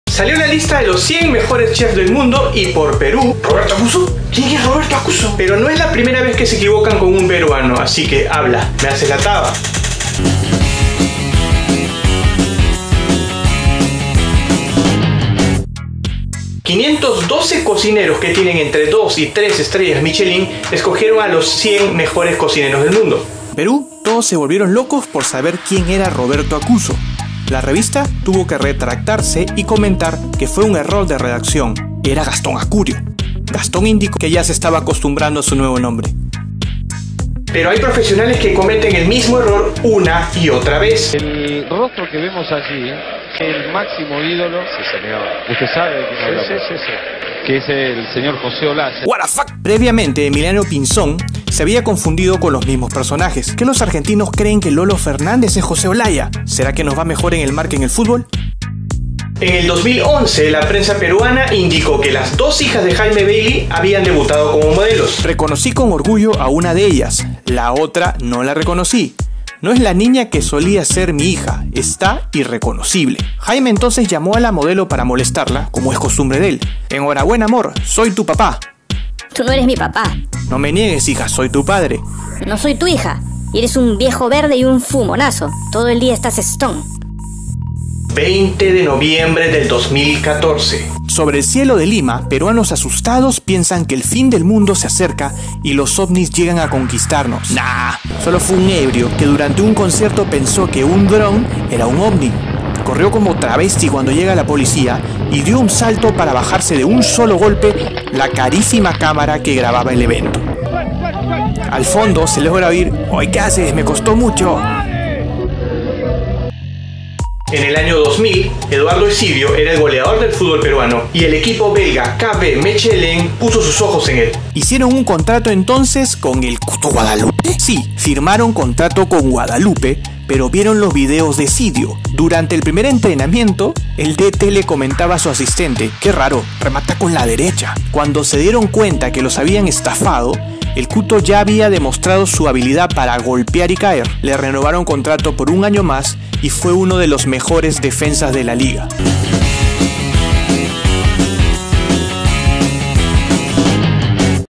Gastón Acurio, le cambiaron de nombre a Roberto Acuso - Me Haces la Taba
A Gastón Acurio, Chef peruano, le cambiaron el nombre por Roberto Acuso al incluirlo en el Top 100 Chef. Ríete con este y otros peruanos que fueron desconocidos.